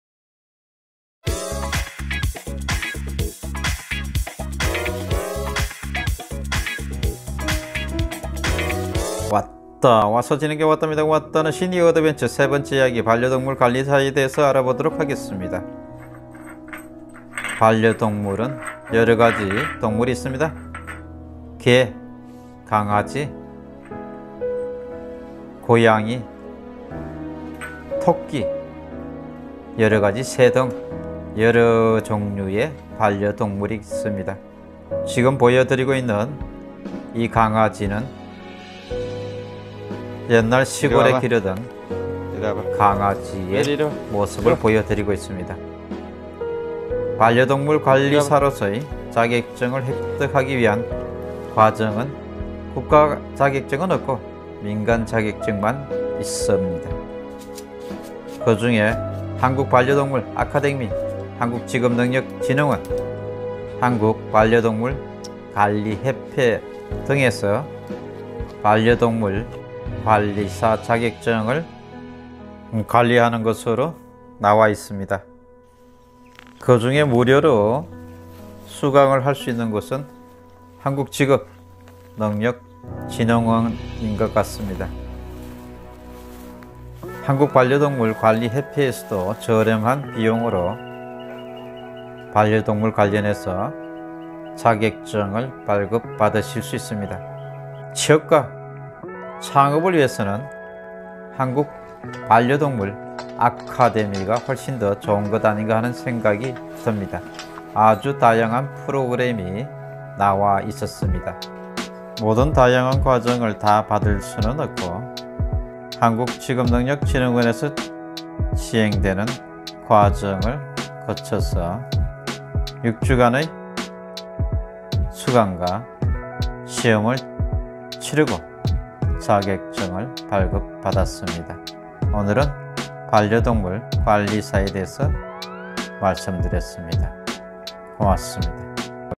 [반려동물 관리사 자격증 따기 ]반려동물에 대한 이해와 관리 및 반려동물보호자와 반려동물과의 올바른 관계를 돕고, 반려동물산업 전반의 활동,Pet Animal~시니어 도전기 3
반려동물의 전반에 대한 이해와 관리를 통한 반려동물보호자와 반려동물과의 관계를 설정하고 이해를 돕는 역할을 할수 있으며 반려동물산업 전반에 걸쳐 활동하는 전문가로서의 취업과 창업 및 프리렌서와 직업을 선택할수있는 자격제도입니다